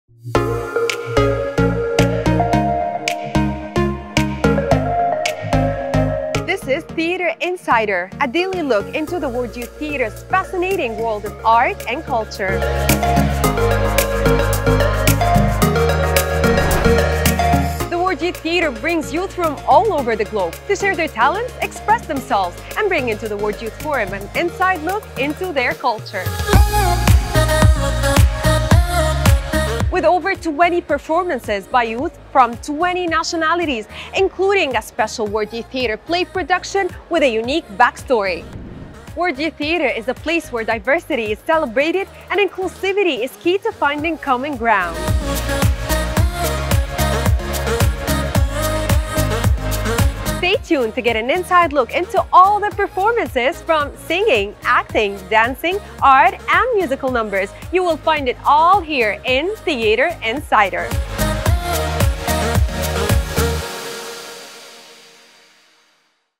The World Youth Theater is back for the second time
The World Youth Theater is back for the second time! World Youth Theater Insider will reveal to you the fascinating world of arts and culture showcased by our youth from all over the world.